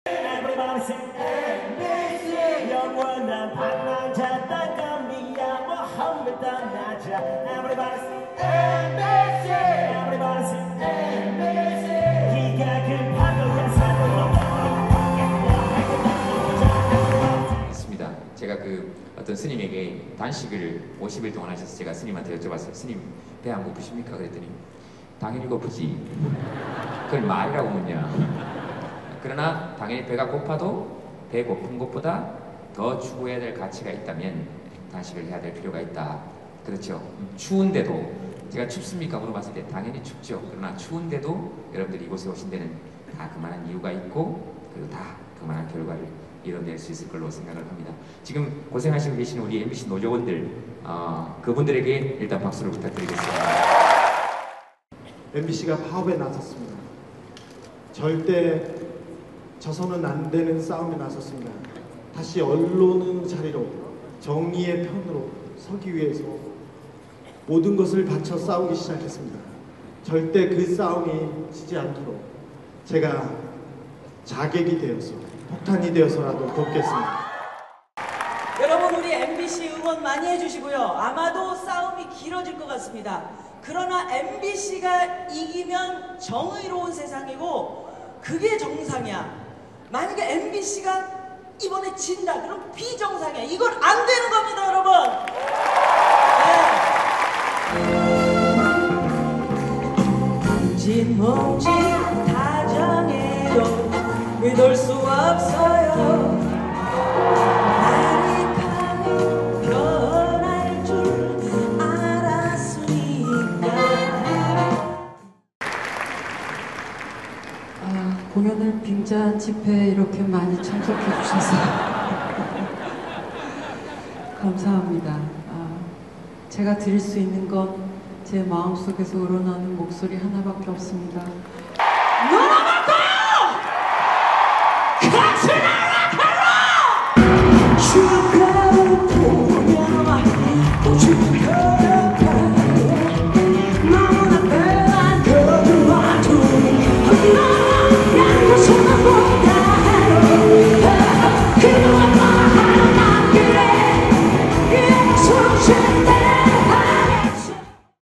으랏차차 MBC 콘서트..한마음으로 "힘내라"
MBC 노조의 파업을 지지하고 응원하기 위한 콘서트 '으랏차차 토크 콘서트'가 열렸다.

17일 오후 서울 장충체육관에서 열린 '으랏차차 토크 콘서트'에는 방송인 김제동, 김미화, 가수 이은미, 작가 공지영, '나는 꼼수다' 주진우, 김어준, 김용민 등이 출연했다.

이날 무대에서 김제동은 "가수 길이 술을 흘리면 나에게 '죄송합니다 형님 피같은 술을 흘려서'라고 말한다"며 "그러면 나는 '하찮은 피를 술에 비교하지 말라'고 말한다"고 말해 관객들의 궁금증을 샀다.

그 이유에 대해 김제동은 "피는 다시 뽑으면 되지만 술을 다시 사야되지 않냐"며 "사장은 다시 뽑으면 되지만 방송은 늘 시청자의 것이어야 한다"고 말해 큰 환호를 받았다.

가수 이은미는 "공연을 빙자한 집회에 오신 여러분들 반갑습니다"라며 "제가 드릴수 있는 것은 제 마음속에서 울어나오는 목소리 하나밖에 없다"고 말해 관객들의 환호를 받았다.

이어 이은미는 멋진 로큰롤 음악무대로 관객들을 열광시키고 관객석을 뛰어다니며 최고의 팬서비스를 보여줬다.

한편 이날 가수 강산, 이한철, 커피머신 등이 출연해 무대를 뜨겁게 달궜다.